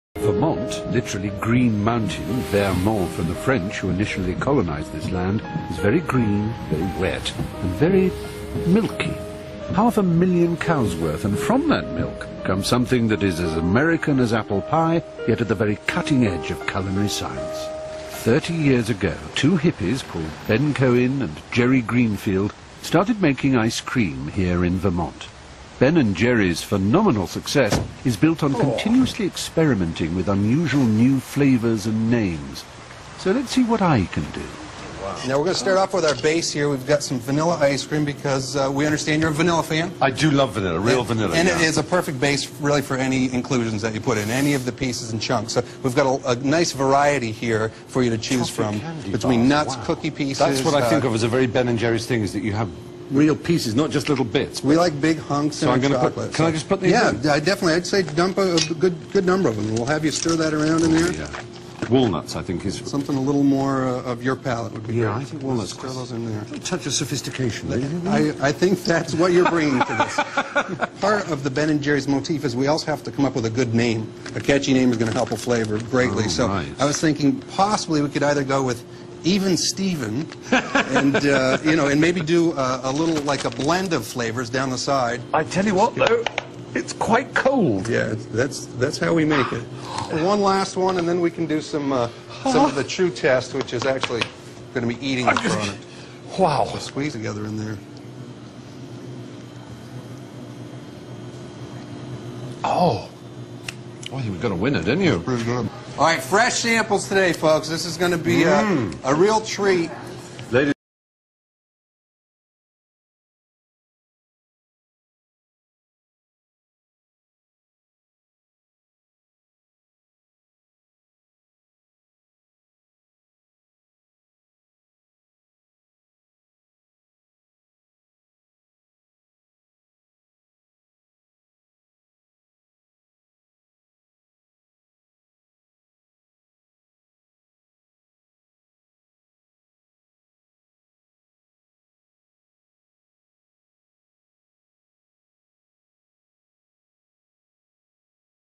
Stephen Fry at Ben & Jerry's
Makes his own ice cream flavour